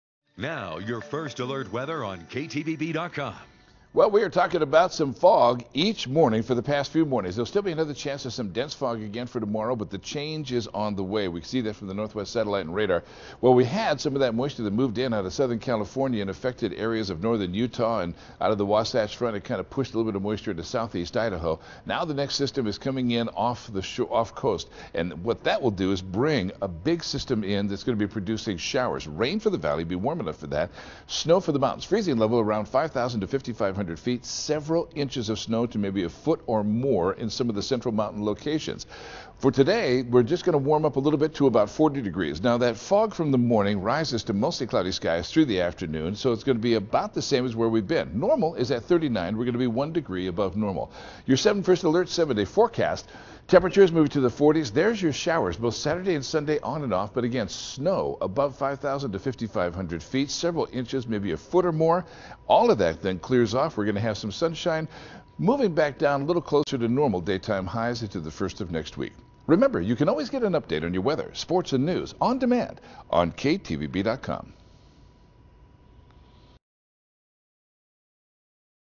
Weather forecast for Thursday, December 5
Larry Gebert says foggy conditions in the morning, clearing by the afternoon with highs in the 40s.